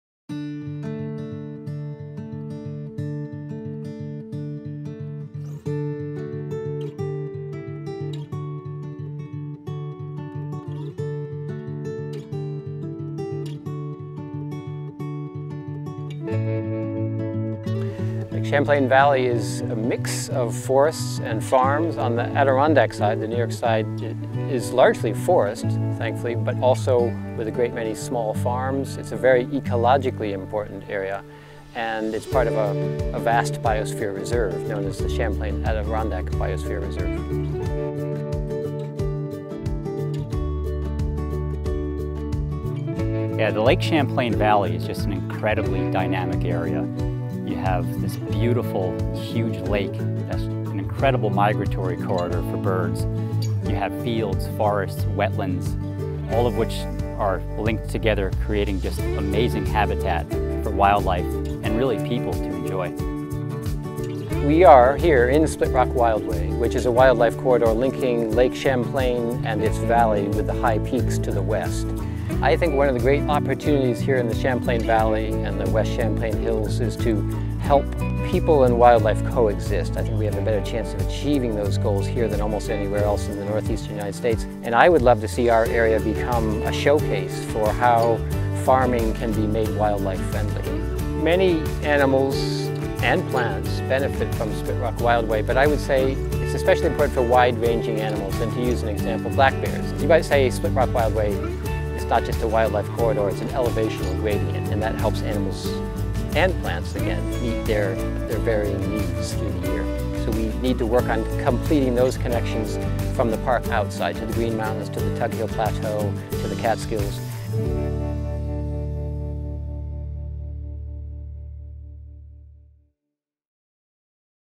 Adirondack Symbiosis: The Wilderness Farmscape of the Lake Champlain Valley
Explore the dynamic habitats of the Lake Champlain Region, where the Adirondack High Peaks meet plentiful farmland and extensive freshwater. It's a place where wildlife, wilderness, and humans coexist, and communities develop with our natural heritage at the heart.